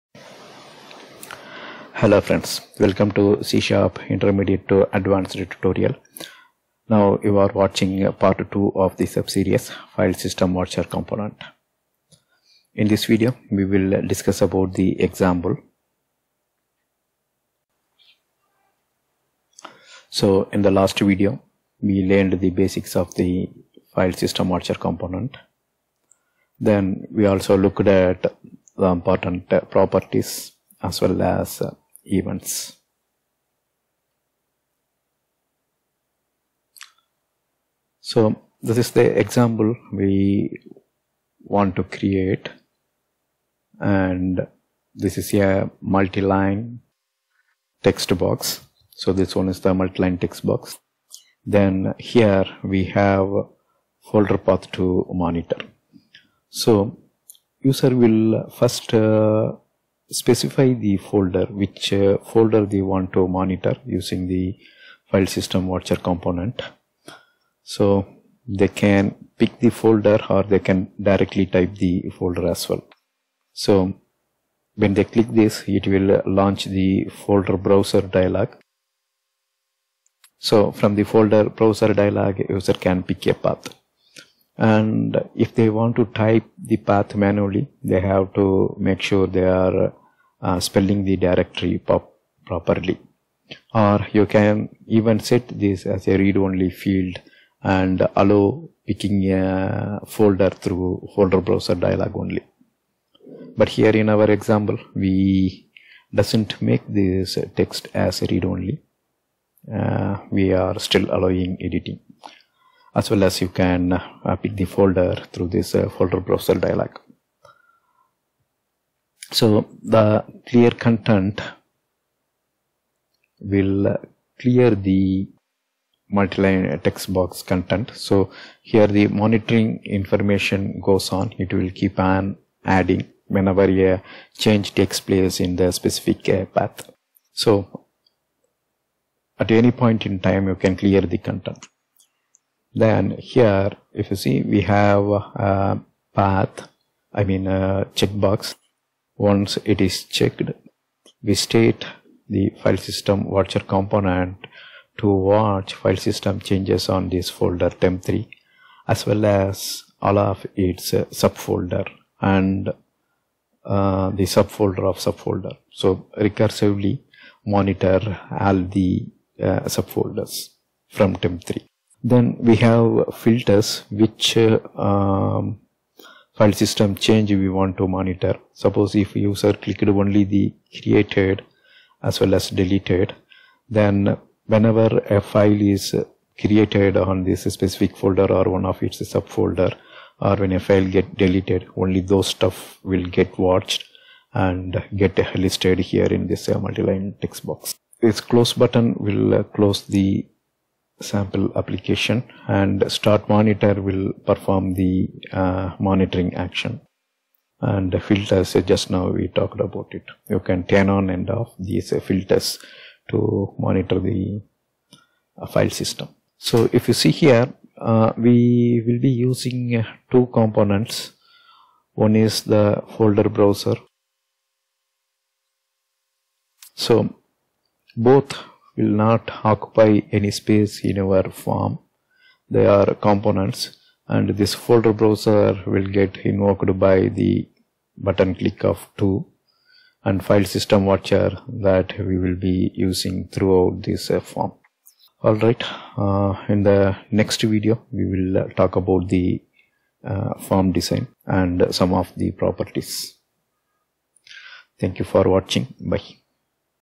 C# FileSystemWatcher | Part 2 - About the Example | C# Advanceed #132
In this C# advanced video, we will talk about FileSystemWatcher example which we create in the next two videos.